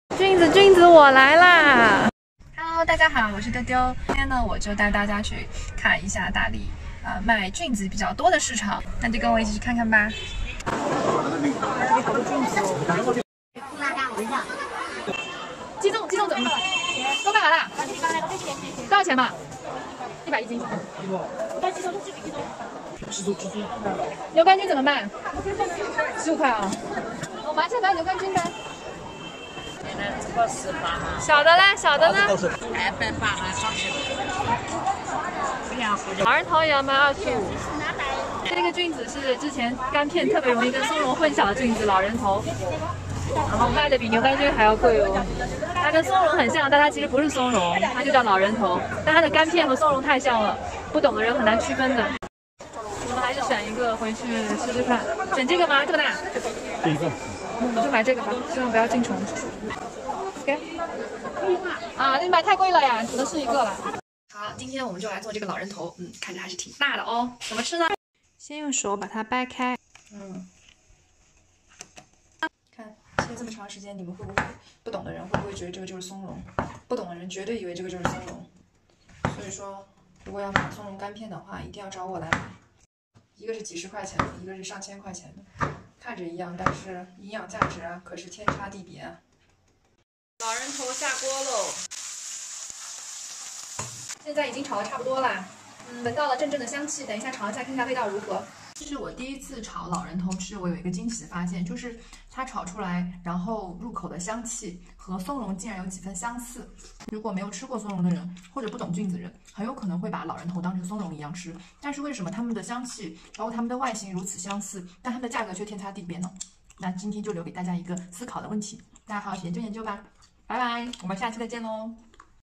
How to identify the real Yunnan's Matsutake mushrooms between the fake ones ?
Come with us to the domestic Yunnan's mushrooms market to find the real Matsutake and cook the fake ones to taste the difference !